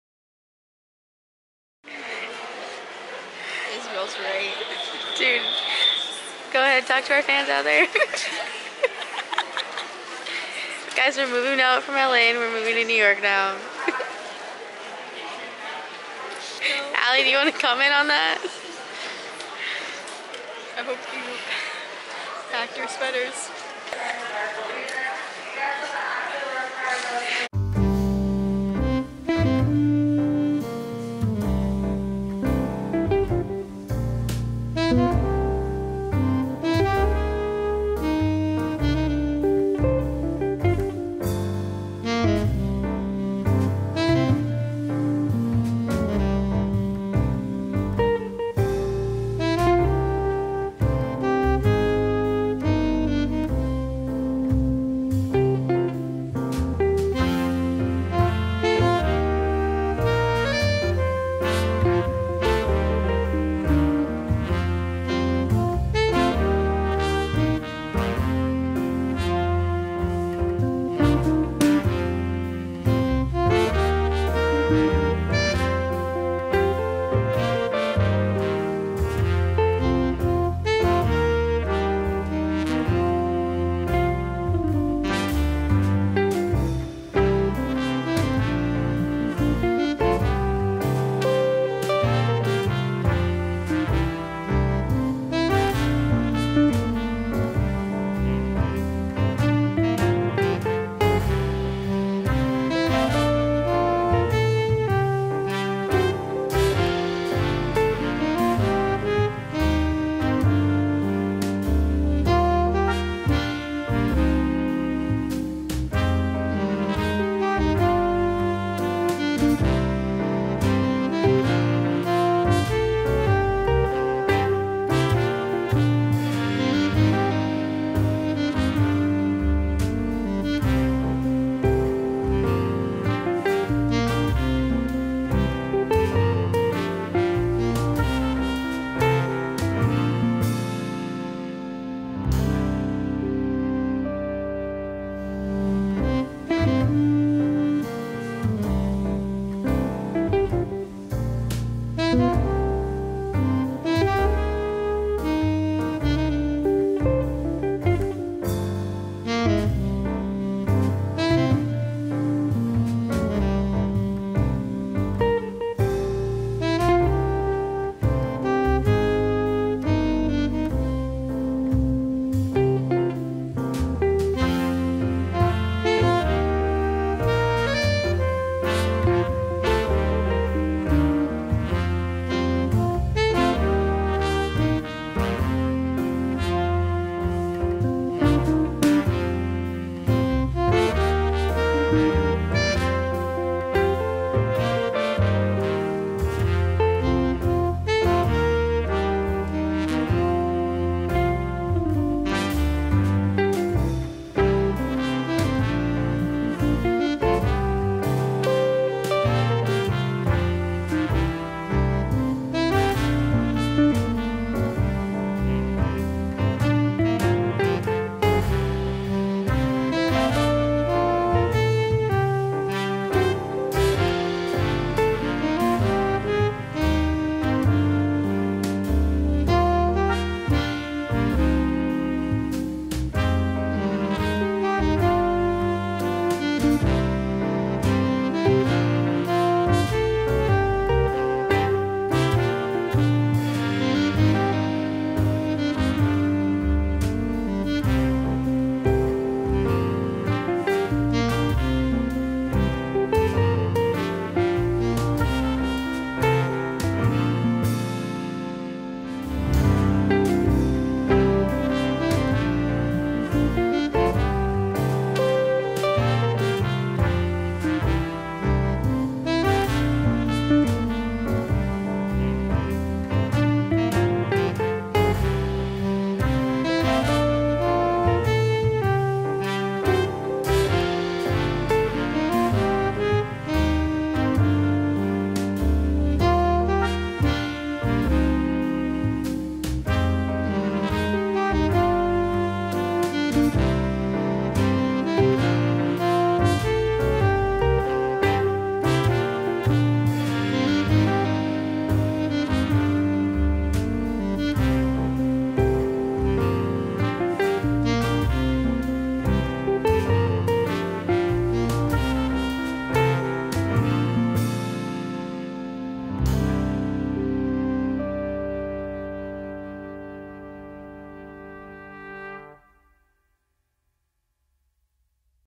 A Trip to New York City
Our first trip together after three years of dating!